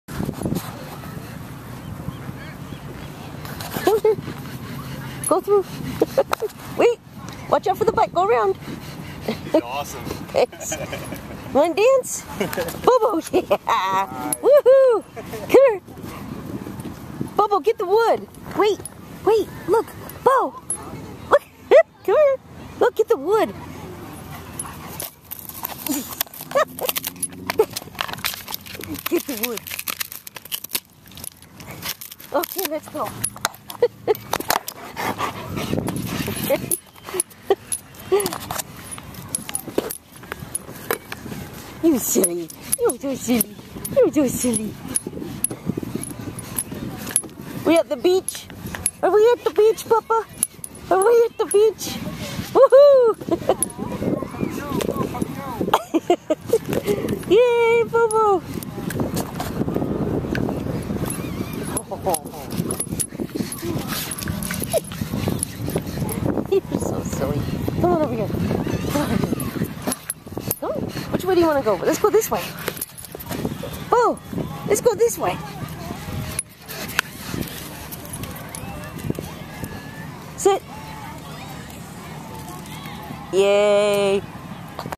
Beach Dance
Just videoing the hilarity that is Borias when he gets to the beach. Recorded on August 11, 2011 using a Flip Video camcorder.